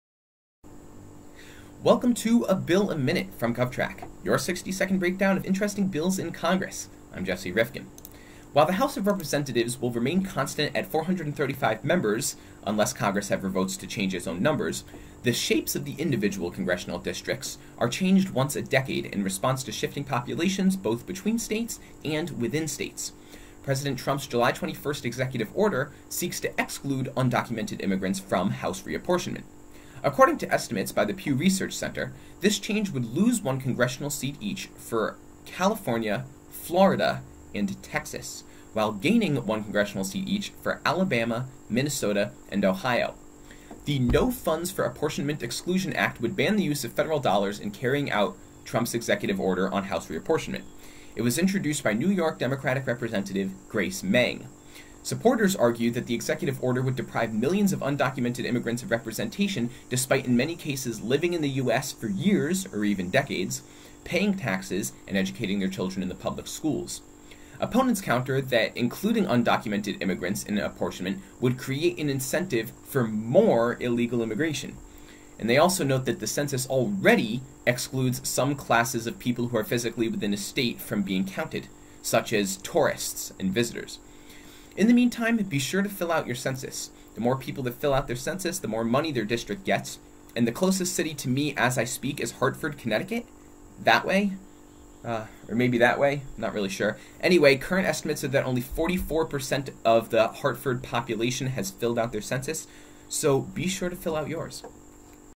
HR2274 No Funds for Apportionment Exclusion Act
In this installment of GovTrack A Bill A Minute we explain the No Funds for Apportionment Exclusion Act and remind you all to fill out the Census if you haven't yet! The better the response rate, the more federal funding flows to your area and the more Congressional representation you get.